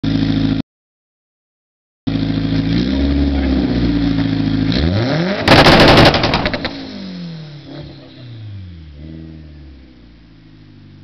2-step!!!
just got off the MUSTANG dyno. 648 AWHP. 34 psi. 480 wtq.

now for the track and 9's.